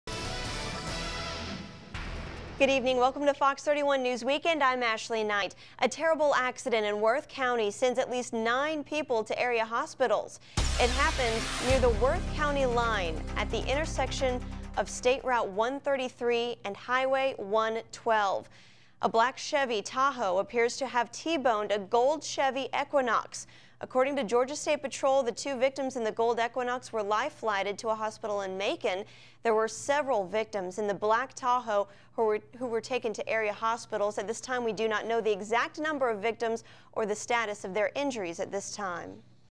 Multiple injuries in Worth County crash
9 people were injured in two-car accident in Worth County.